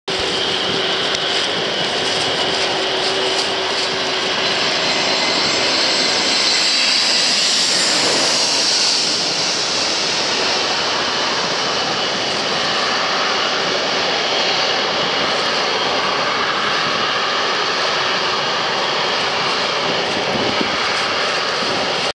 長榮航空 Eva Air A321-211(B-16201) BR-155 金浦(GMP)→松山(TSA) landing
Eva Air flight 155 from RKSS landed on RWY 10, RCSS on Oct. 12th, 2018.